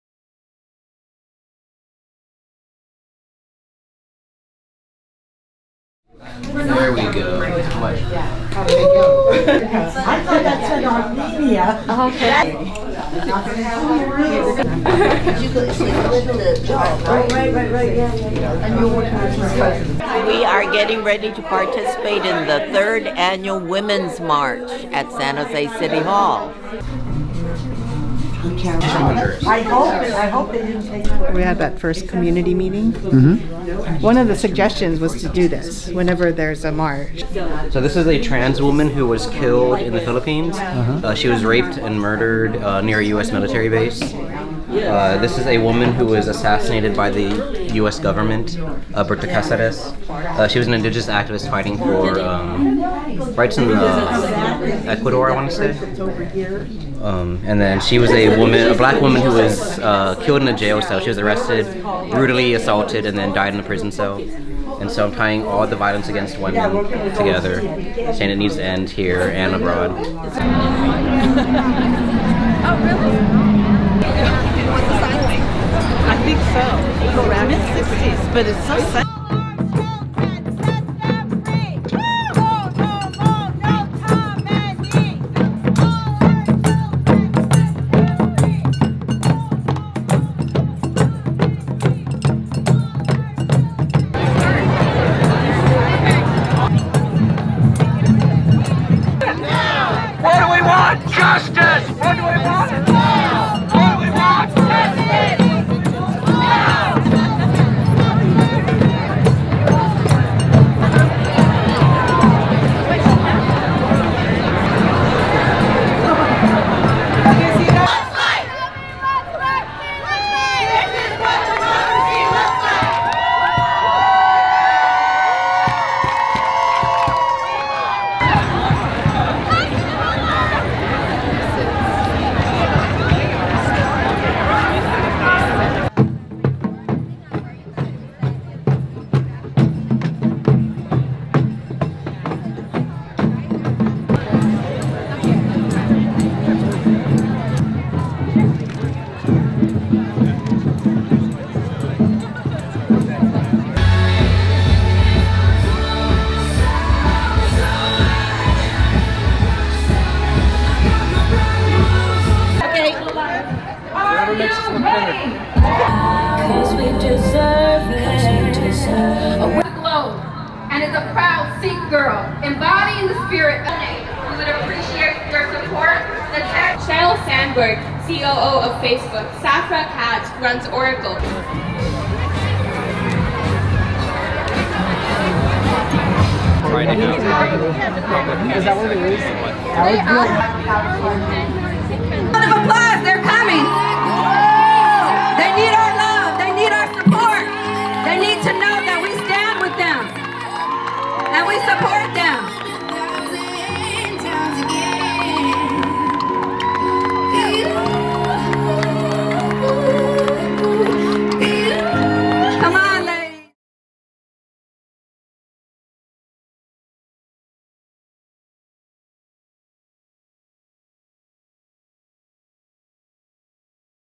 Never Again is Now! @ Women's March 2019
Nikkei women and men led by San Jose Nikkei Resisters and San Jose JACL took to the streets for the third  Women's March in San Jose, CA on January 19, 2019.